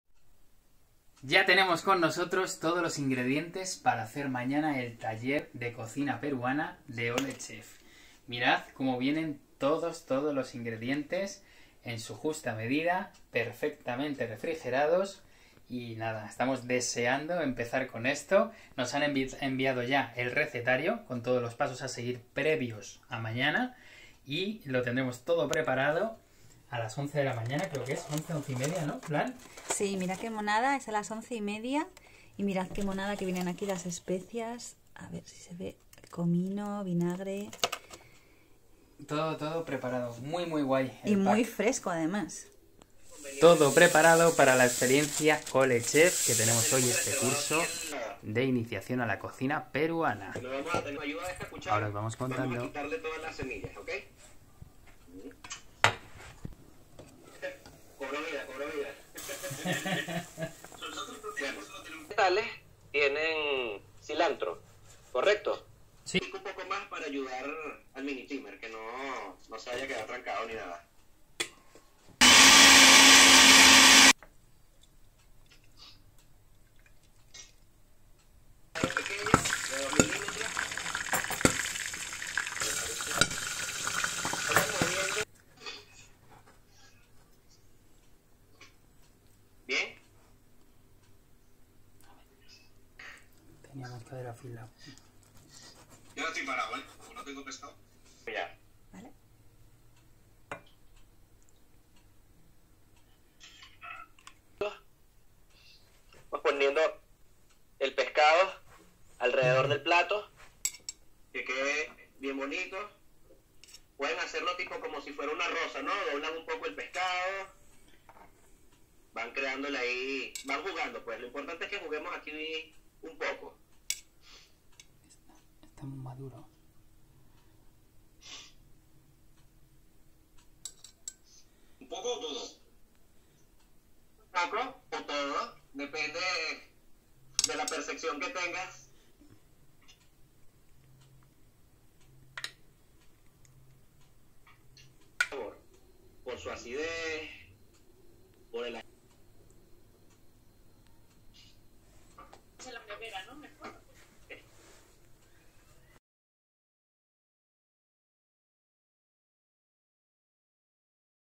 TIRADITO DE CORVINA | Taller de cocina peruana I
La semana pasada nos apuntamos a un taller de cocina con Oh Le Chef.
Podíamos elegir diferentes tipos de cocinado y nos llamó la atención el taller peruano, pero podéis entrar
te mandan todos los ingredientes a casa, todo lo que necesitas y el resultado es excepcional!